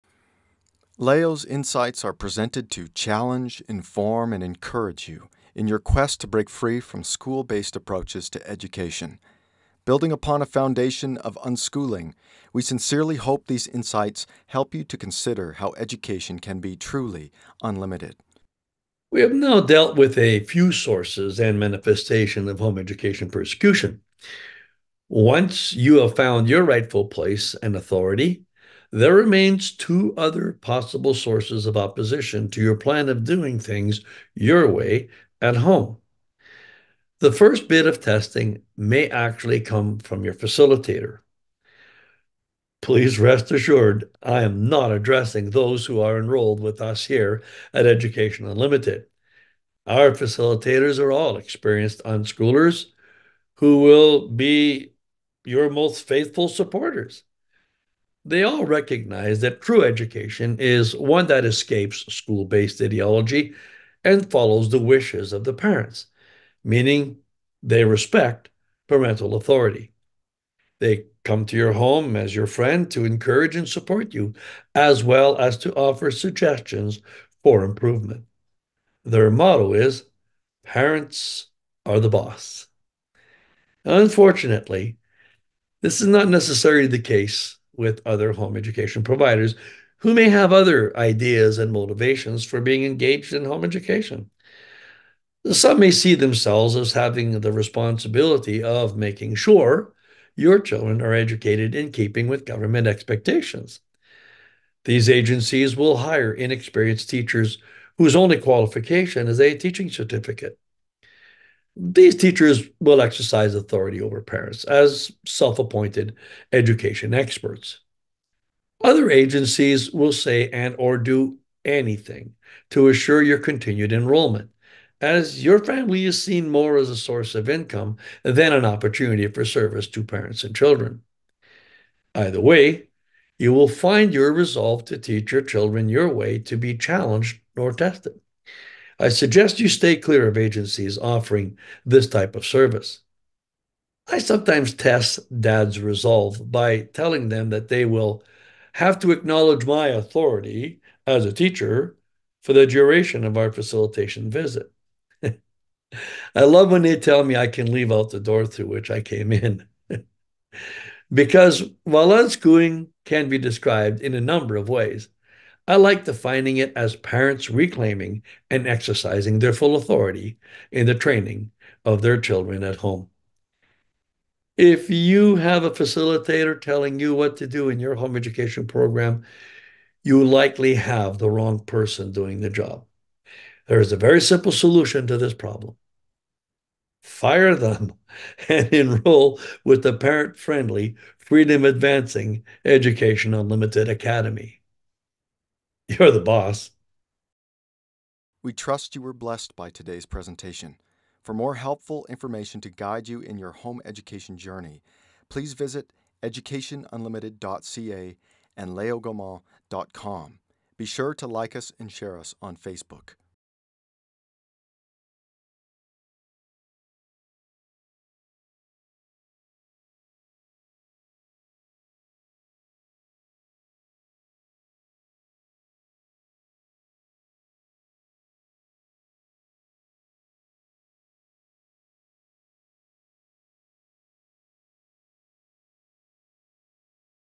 Testing Home Education - Part 6: Back to the Basics (Part 25) by Léo Gaumont
In this video, Léo Gaumont explains that if you have a facilitator telling you what to do in your home education program, you likely have the wrong person doing the job.